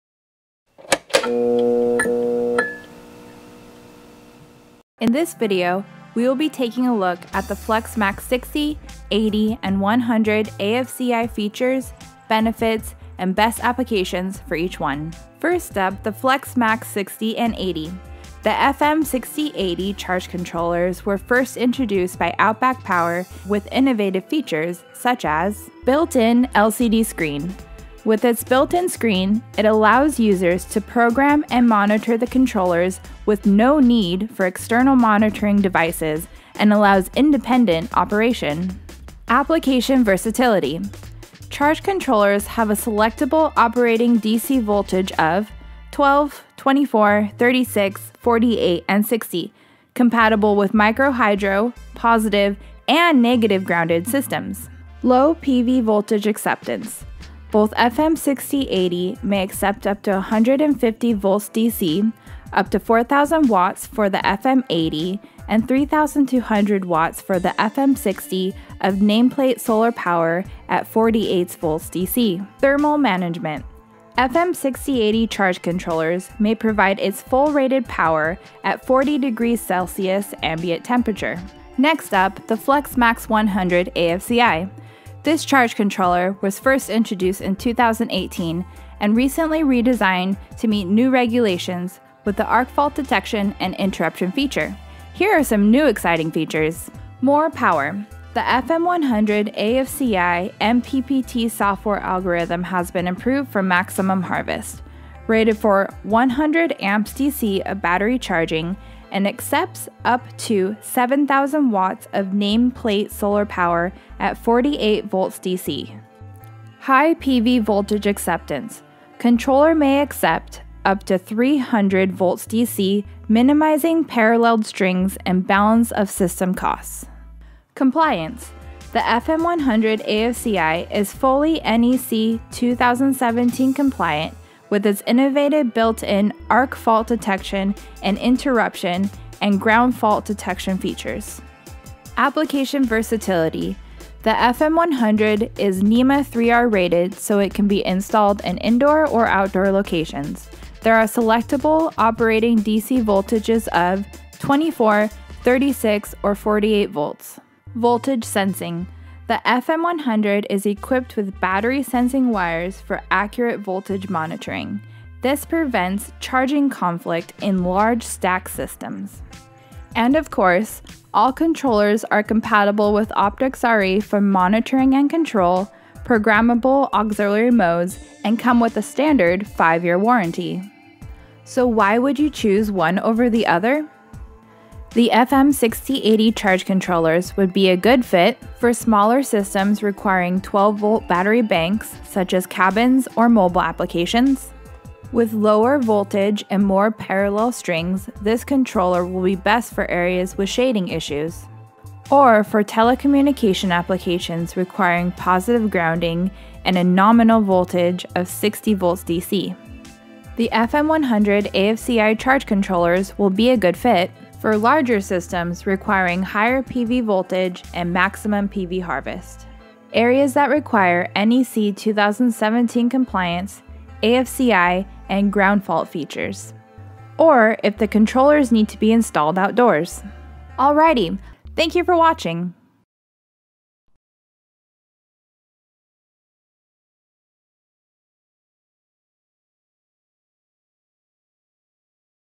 FLEXmax 80/100 AFCI: Comparison
Learn about the differences and best uses on OutBack Power's charge controllers; FLEXmax 60/80 and 100 AFCI.